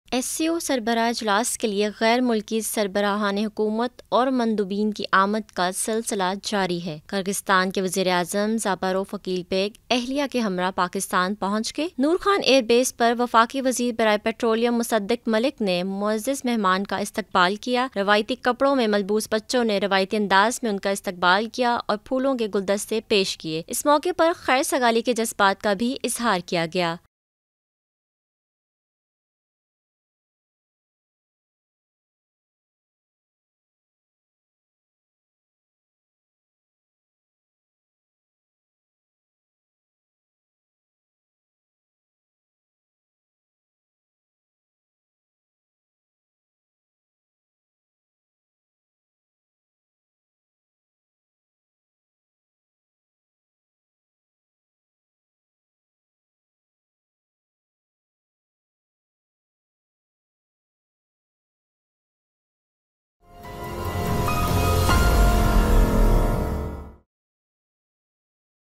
Kyrgyz PM arrives in Pakistan to attend 23rd SCO Summit SCO| APP
Kyrgyz PM arrives in Pakistan to attend 23rd SCO Summit

ISLAMABAD, Oct 15 (APP): Prime Minister of the Kyrgyz Republic Akylbek Japarov arrived to attend the twenty-third meeting of the Council of Heads of Government (CHG) of the Shanghai Cooperation Organization (SCO).
Federal Minister for Petroleum, Dr. Musadik Malik warmly received the distinguished guest along with his delegation at Noor Khan Airbase.